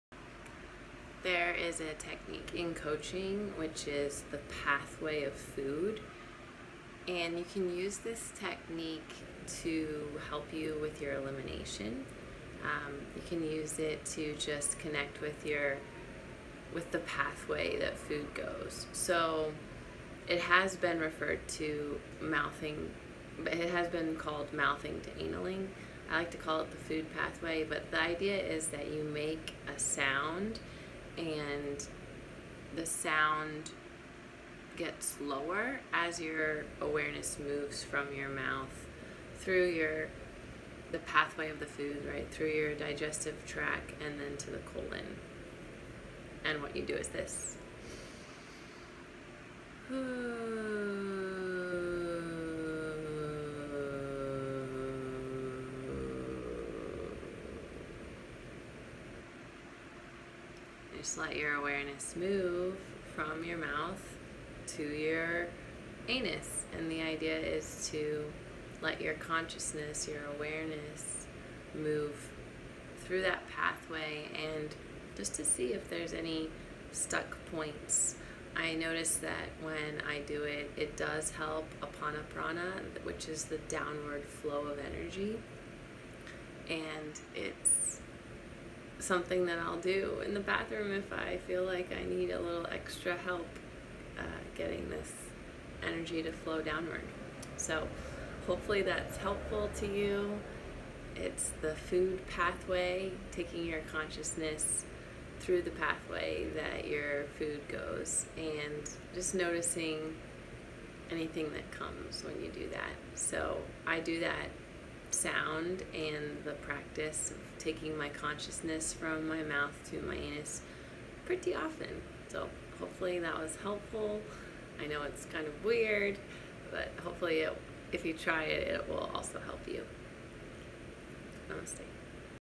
Pathway of Food
The practice is taking your consciousness with the sound “who” from your mouth to your anus. Cate Stillman introduced me to this practice. It helps!